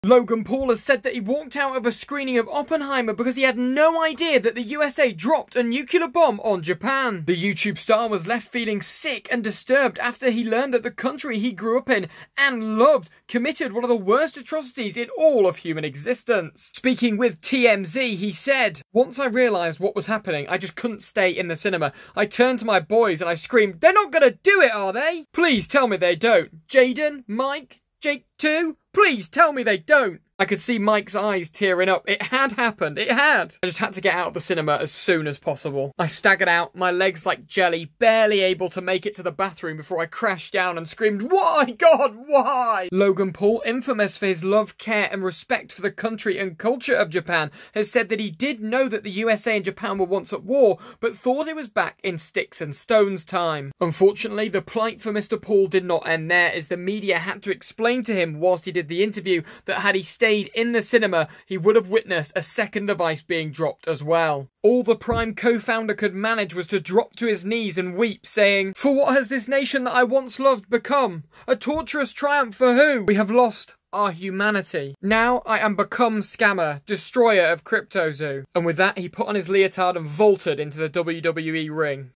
Why Did Logan Paul Walk Out Of Oppenheimer?
Logan Paul said that he walked out of his screening of Oppenheimer because he never realised the United States dropped a nuclear bomb on Japan.

The YouTube star & Prime drink co-founder has said the information has devastated him to a point where he is barely able to get into the WWE ring or scam people like with CryptoZoo.